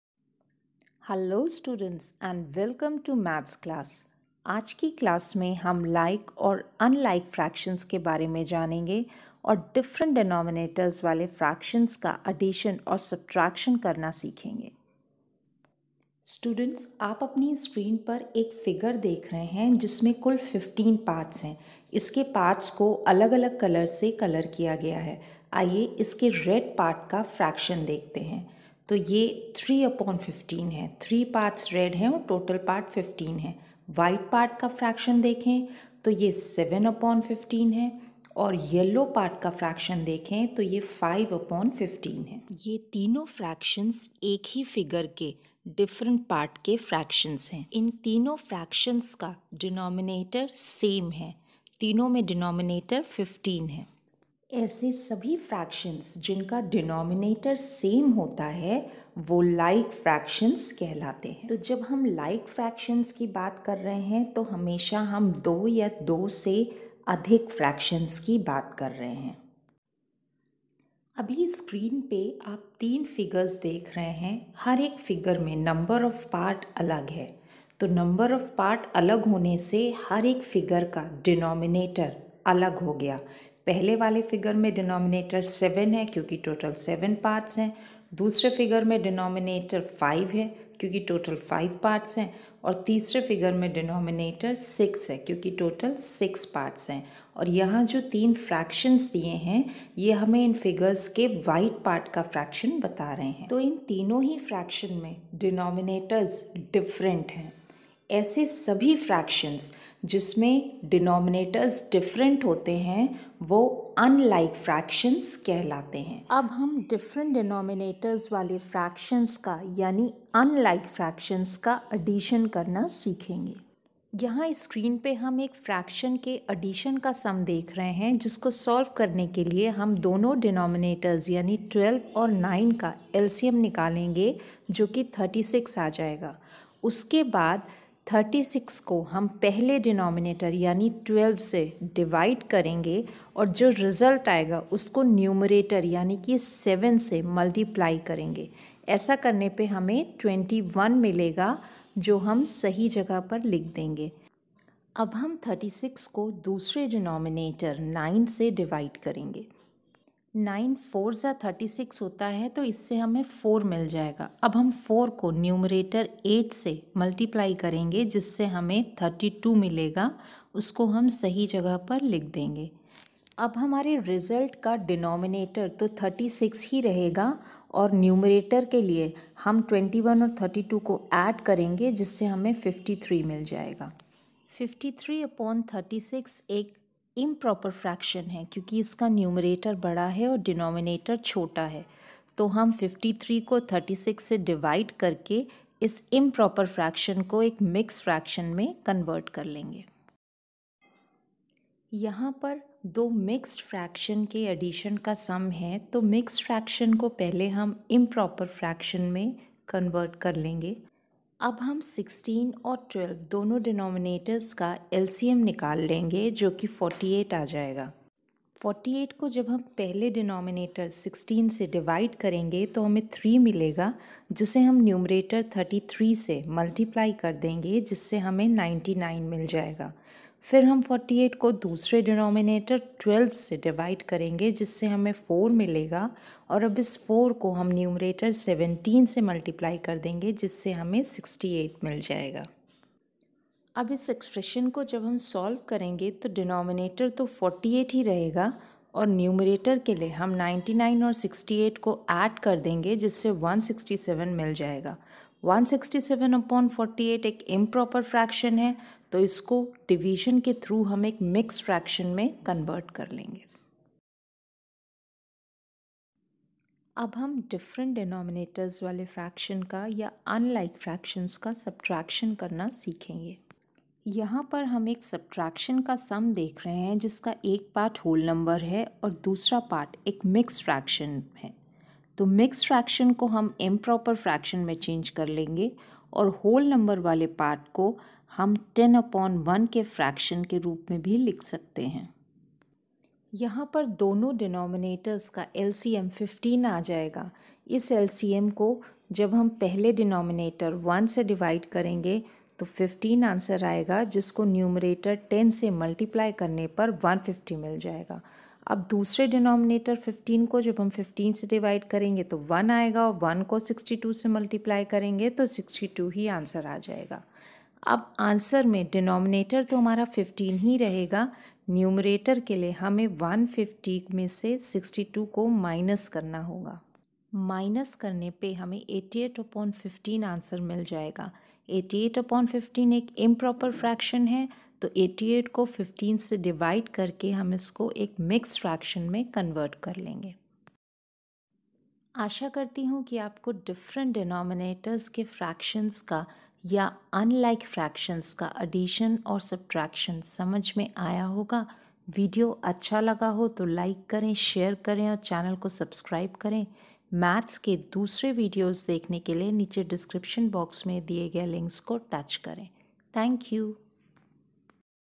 Class 4 gintara Like and Unlike Fractions Addition and subtraction of unlike fraction
Like and Unlike Fractions
Addition and subtraction of fractions with different denominators